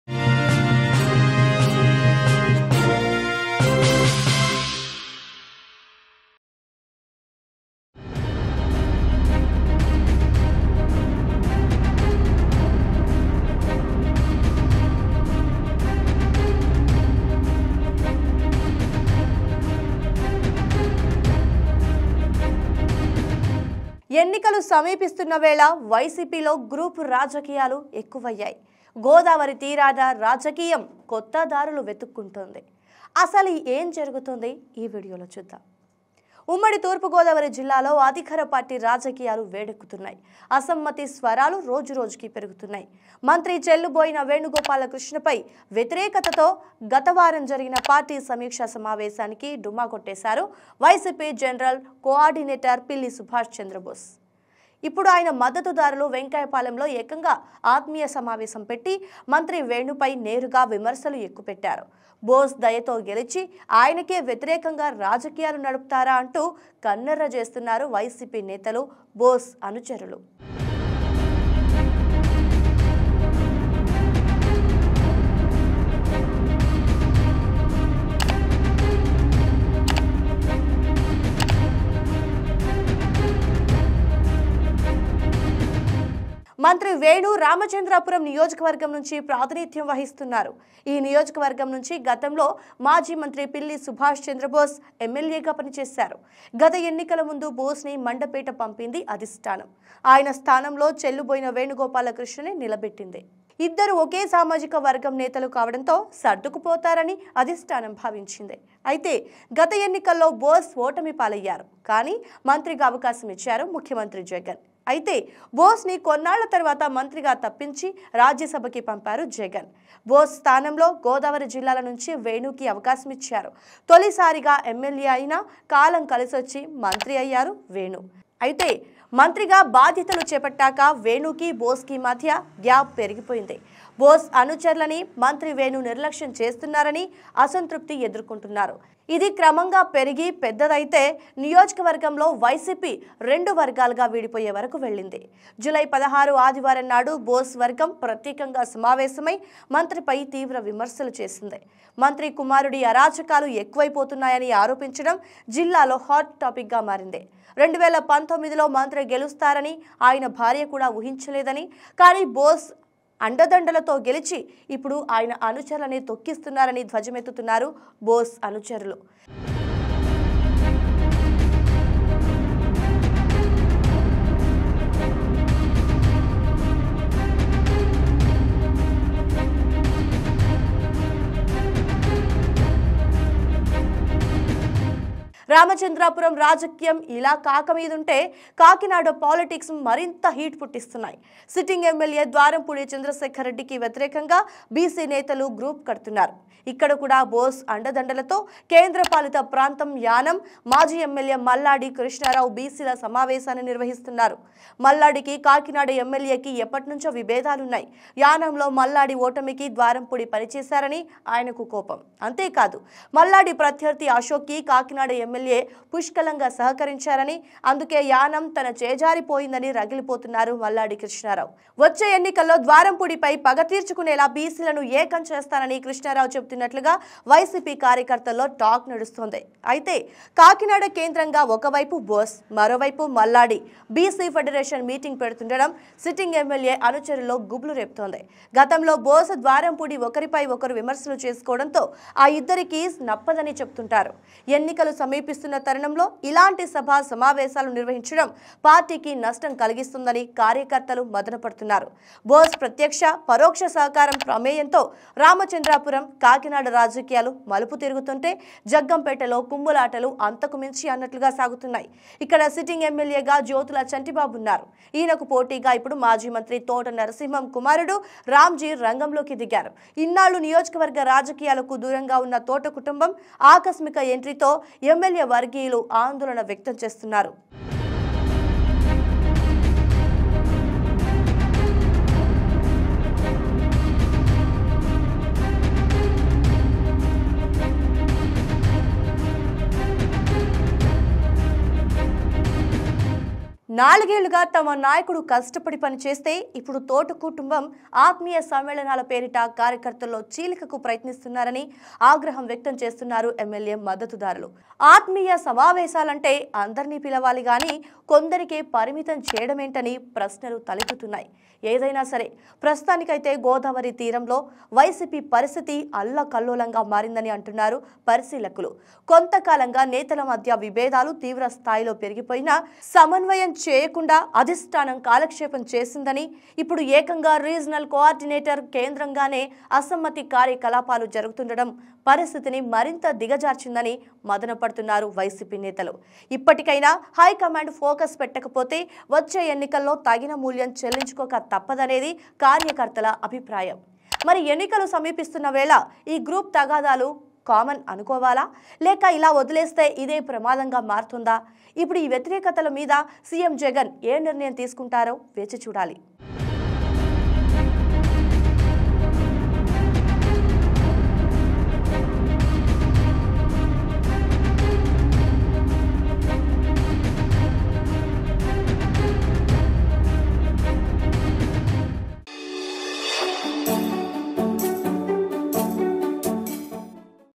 YSRCP లో వర్గపోరు..! | Cold War Between Pilli Subhash Chandra Bose And Minister Venu | Socialpost TV
YSRCP లో వర్గపోరు..! | Cold War Between Pilli Subhash Chandra Bose And Minister Venu | Anchor Girija | Socialpost TV

Hello Friends,